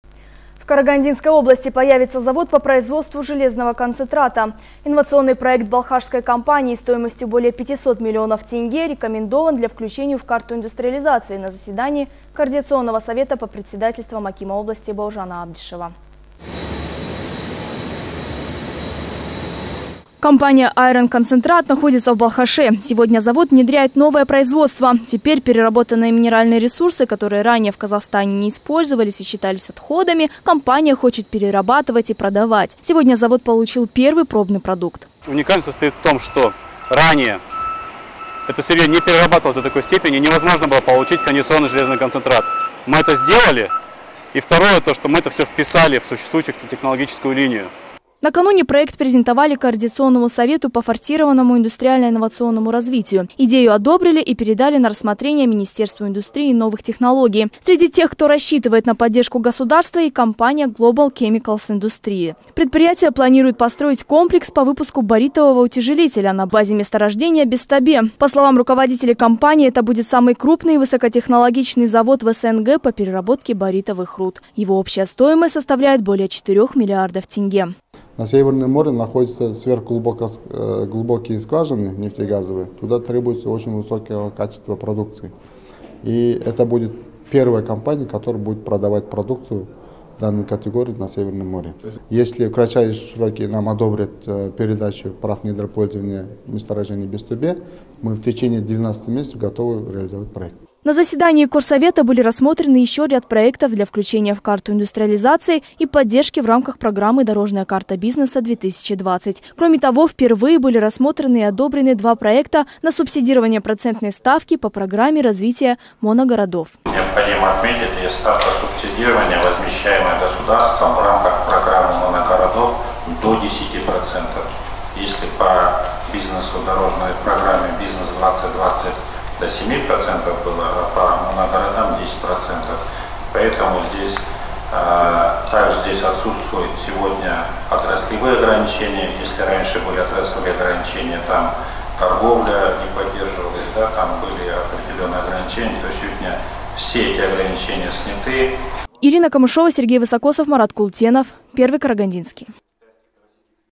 Завод по производству железного концентрата в КарОбл
В Карагандинской области появится завод по производству железного концентрата. Инновационный проект стоимостью более 500 млн. тенге Балхашской компании  рекомендован для включения в карту Индустриализации на заседании координационного совета под председательством акима области Бауржана Абдишева.